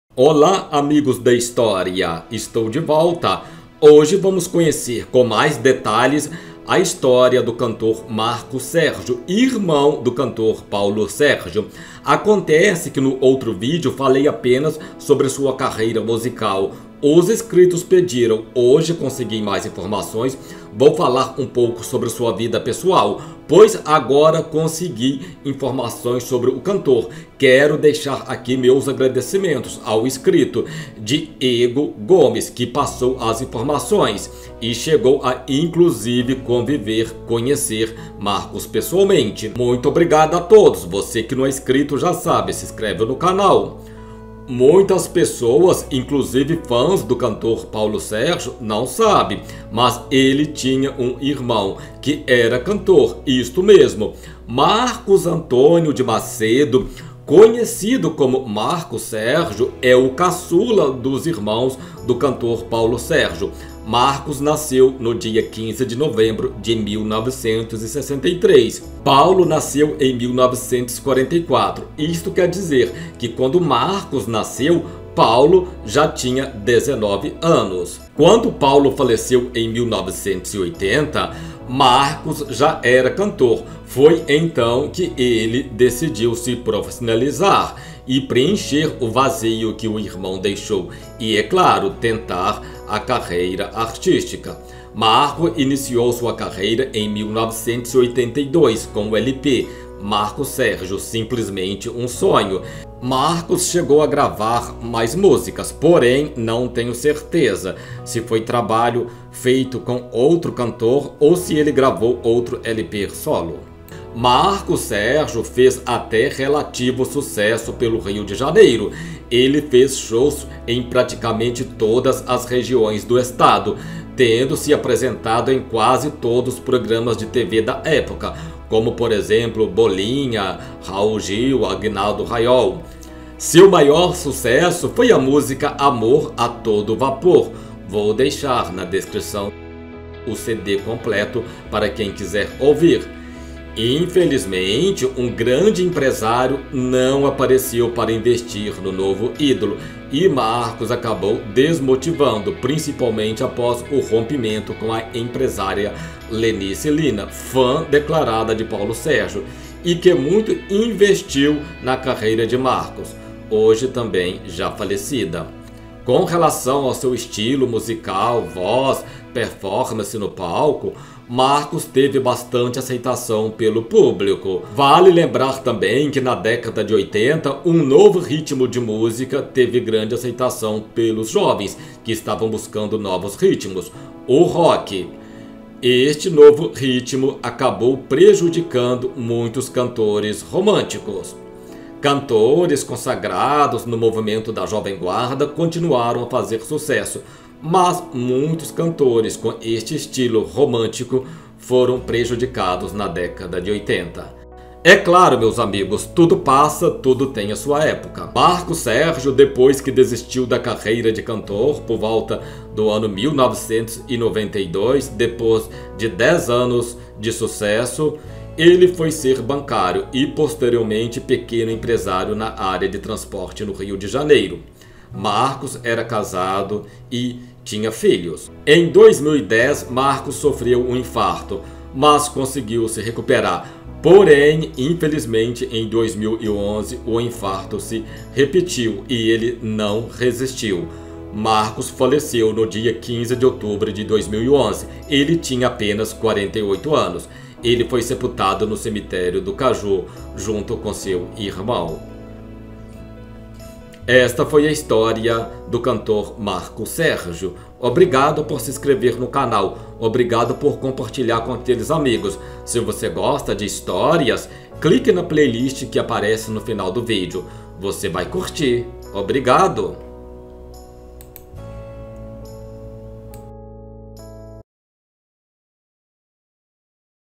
A história com mais detalhes do cantor irmão de Paulo Sergio . Marcos Sergio 1963+2011 vídeo 02 .
Contatos para locução ,apresentação de eventos